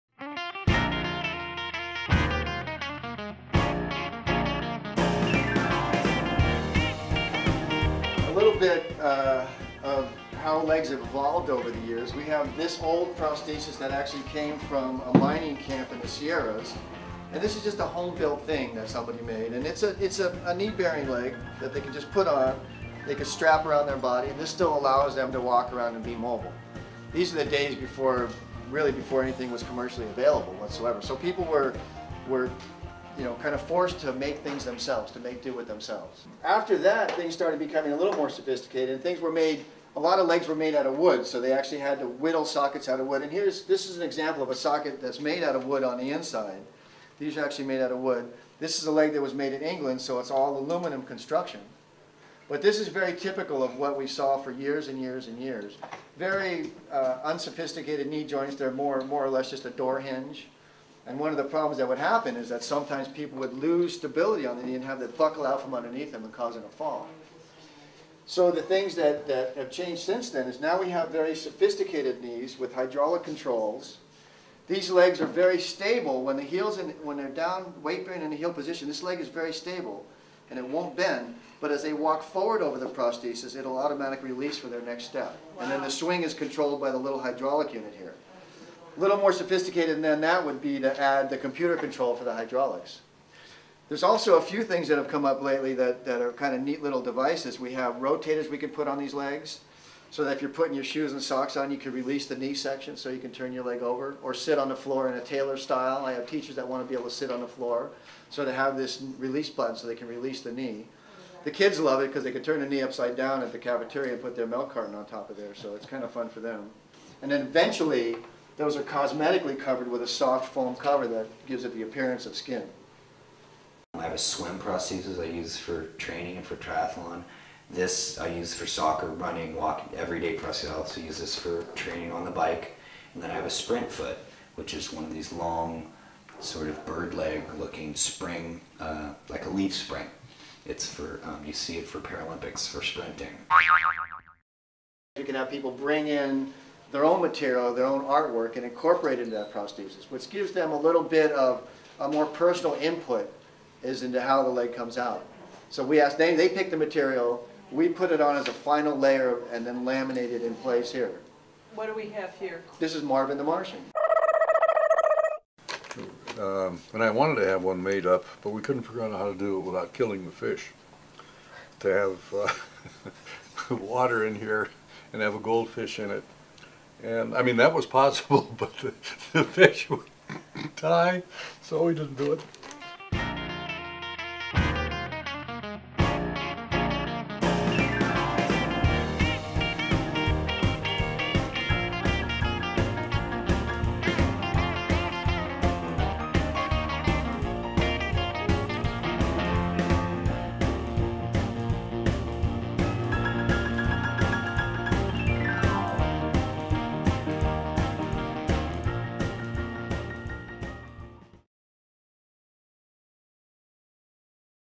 Stumps R Us, an Irreverent Amputee Support Group, Part 5: Best Foot Forward
From pirates' "peg legs" and hollow metal legs of yesteryear to modern carbon fiber springs and computerized hydraulics, the technology of lower-limb prosthetics has made great strides. Today amputees even have the option of decorating their prosthetic limbs, going wherever their imagination takes them. Here's a brief look at how prosthetic limbs have evolved over the years.